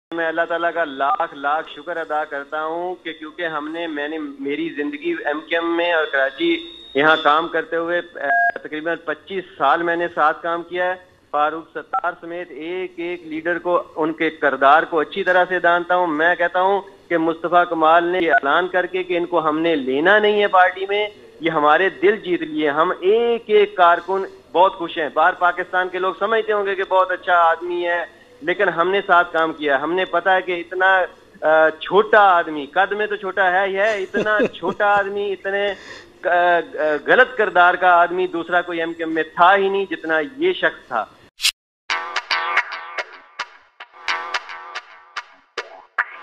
Iftikhar Randhawa-PSP Grilled Farooq Sattar || Part-2
PART-02:
Former MQM leader & close companion of MQM Chief, Altaf Hussain, Mr. Iftikhar Akbar Randhawa grilled MQM's Farooq Sattar over MQM-PSP conflict.